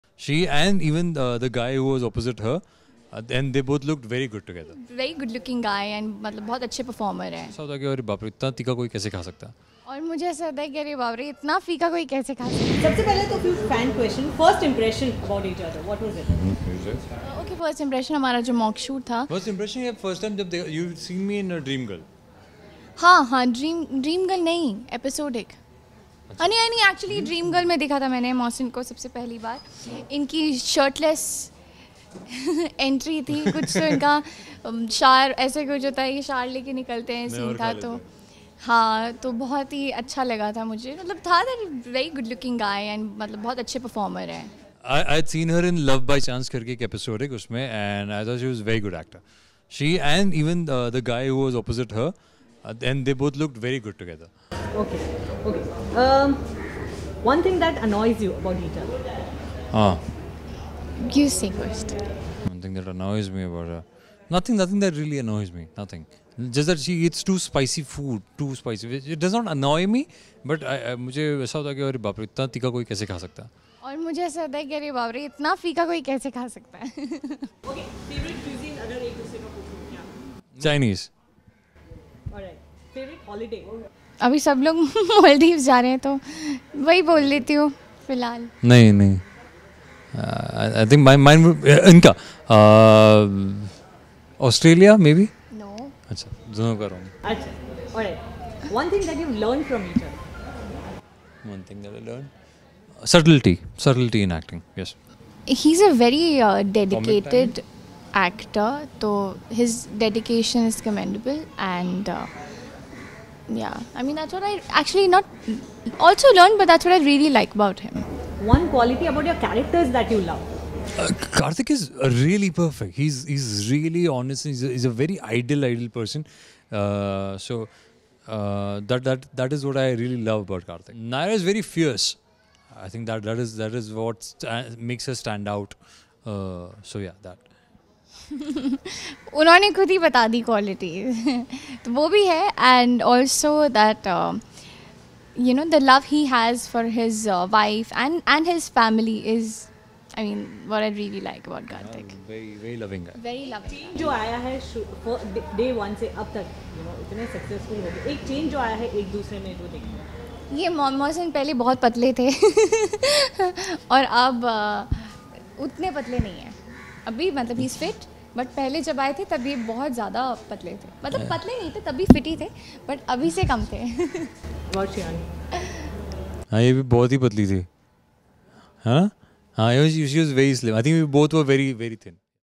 Shivangi Joshi & Mohsin Khan Reveal FUN SECRET About Each Other | Exclusive Interview
In an exclusive interview with TellyMasala, Shivangi Joshi and Mohsin Khan reveal some inside secrets of each other like & dislikes, The duo was loved by their fan in the serial Yeh Rishta Kya Kehlata Hai, as Naira and Kartik.